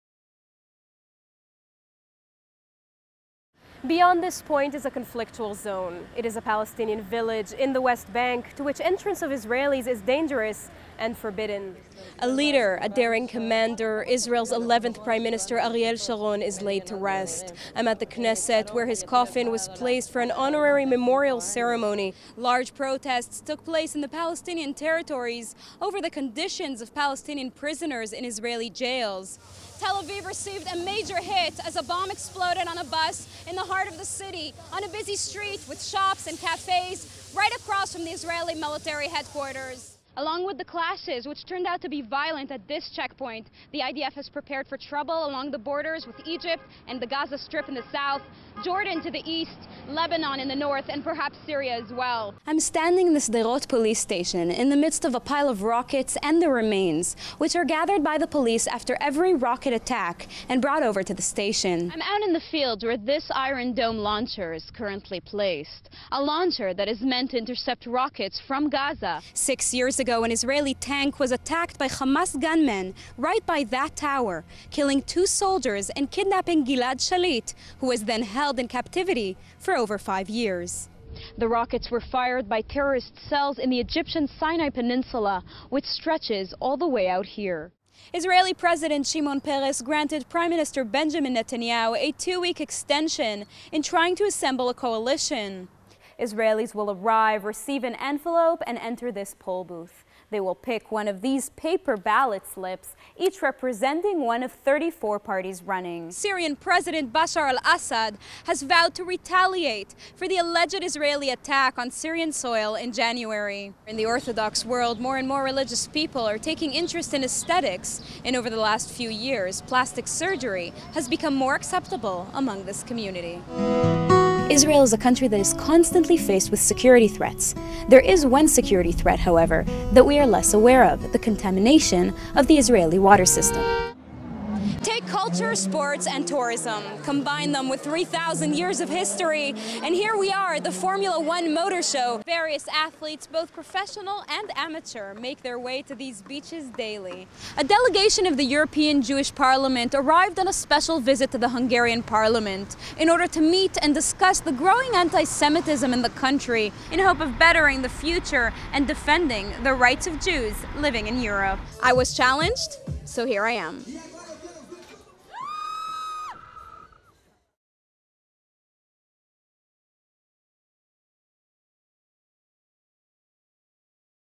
Sivan Raviv - TV Journalist Reel - 2014
Watch TV reporter Sivan Raviv in several on-air segments of her broadcast TV stories - from Israel and the Palestinian territories. Sivan covers geopolitical events, conflict, military operations, economics and human-interest feature stories.
Sivan has a broadcast journalism Masters degree from Columbia University. She speaks English, Hebrew, French and basic Arabic.